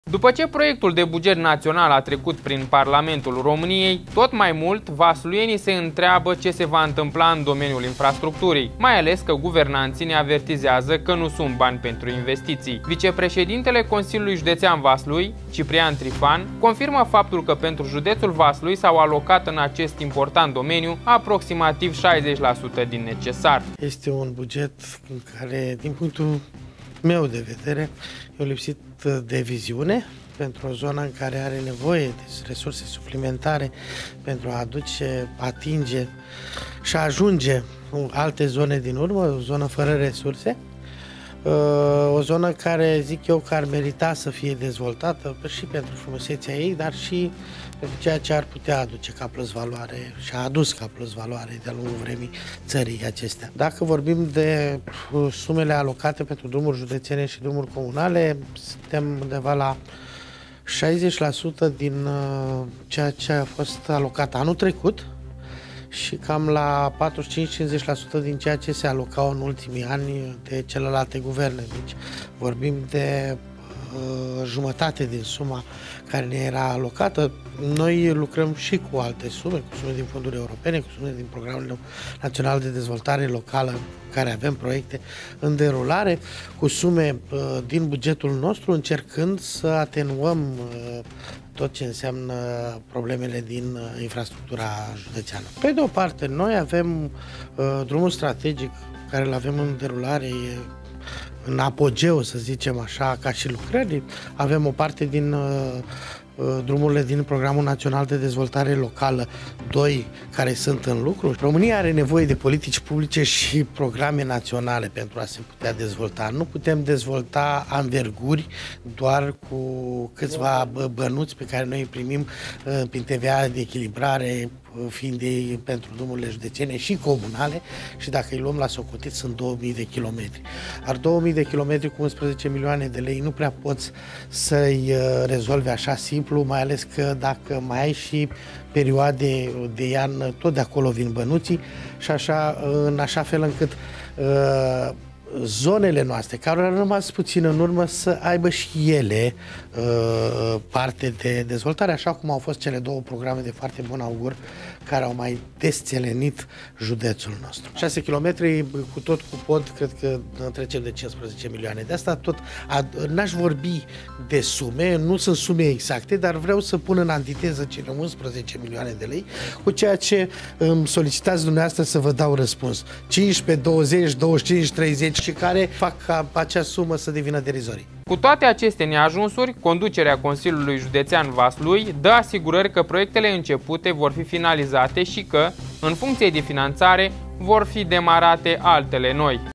INTENTII FOARTE BUNE, BANI SA FIE!
După ce proiectul de Buget Național a trecut prin Parlamentul României, tot mai mult, vasluienii se întreabă ce se va întâmpla în domeniul infrastructurii, mai ales că, guvernanții ne avertizează că nu sunt bani pentru investiții.